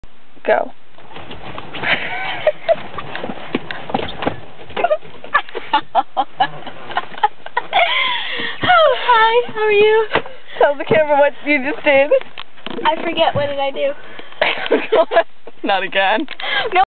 ISABELLE HIT HER HEAD!
Isabelle was attacking the horn with her head!